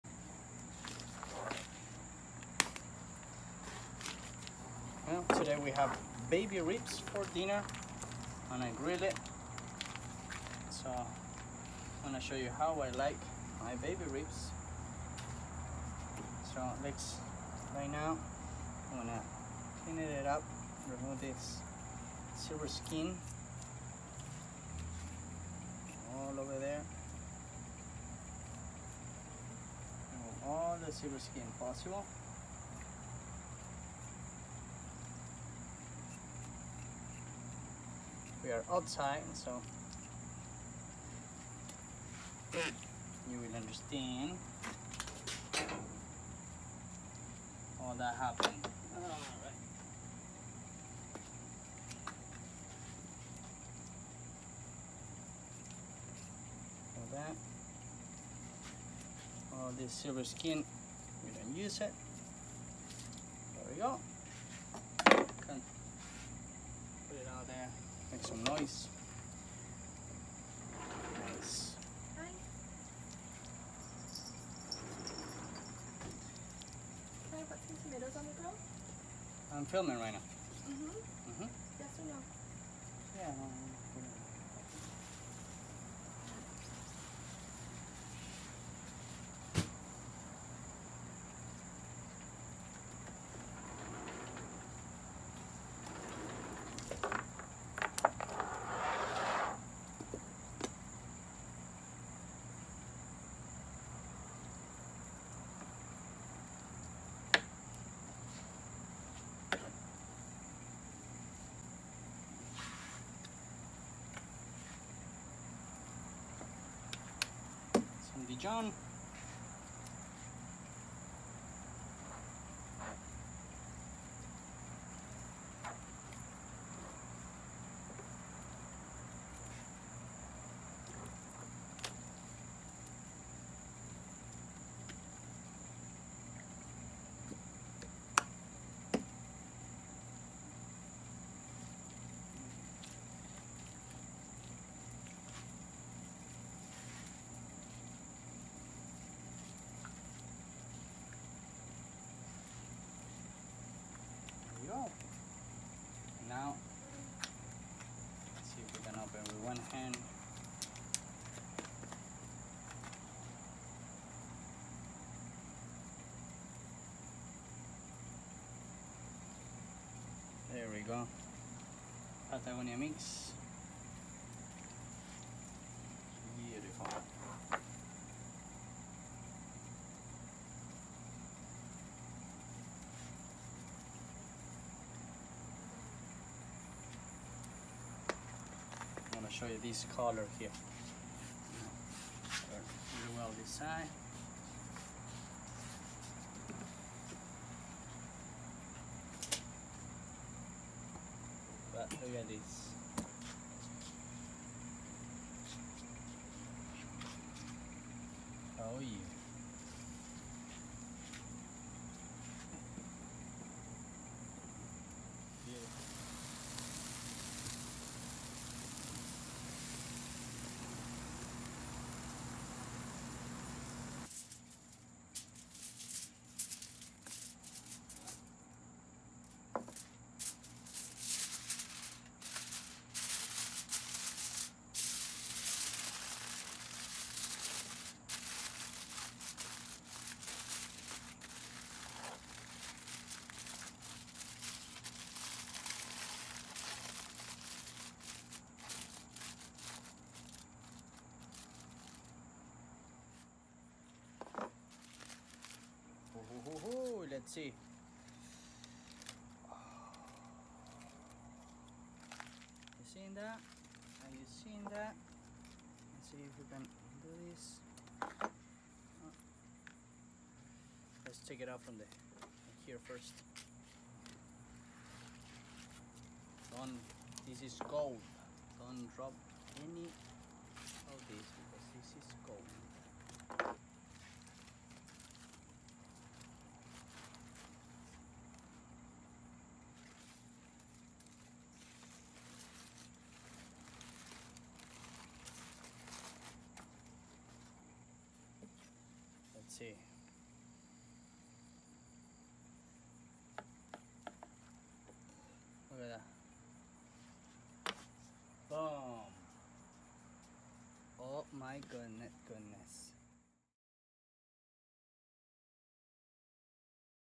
Baby Back Ribs with Patagonia Mix!!
Here is how I like to make my ribs!
How do you make yours??

Follow for recipes @nativocs
Follow our Chef @argentinechef